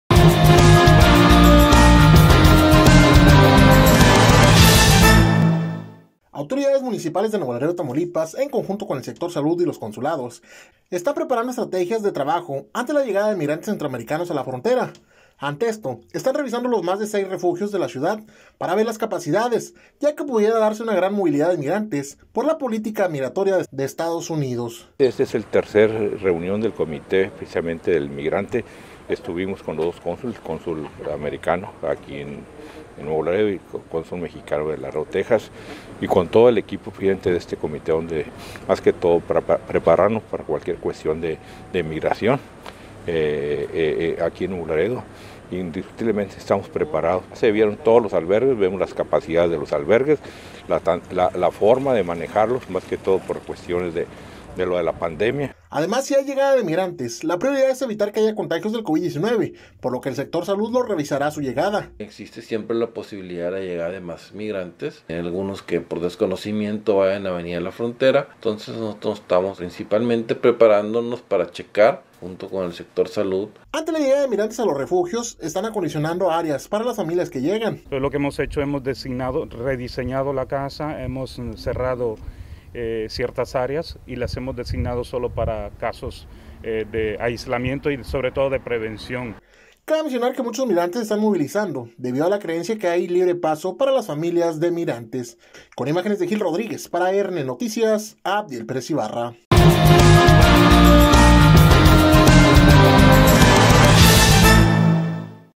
Prepara Nuevo Laredo estrategia ante problemática migrante
Autoridades municipales de Nuevo Laredo Tamaulipas en conjunto con el Sector salud y los consulados, están preparando estrategias de trabajo ante la llegada de migrantes centroamericanos a la frontera.